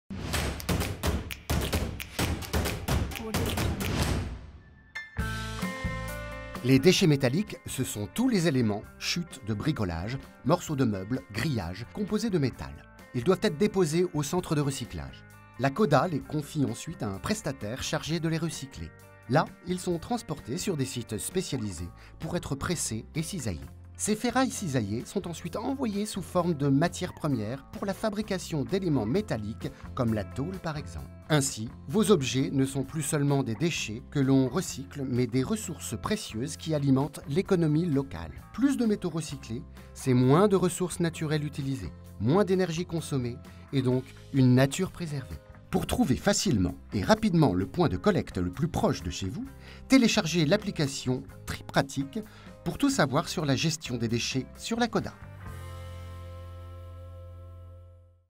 Que deviennent vos déchets une fois triés ? Les déchets métalliques
Que deviennent vos déchets une fois triés ?
Les déchets métalliques, ce sont tous les éléments, chutes et bricolage, morceaux de meuble, grillages composés de métal. Ils doivent être déposés au centre de recyclage.
La CODAH le confie ensuite à des prestataires chargés de les recycler.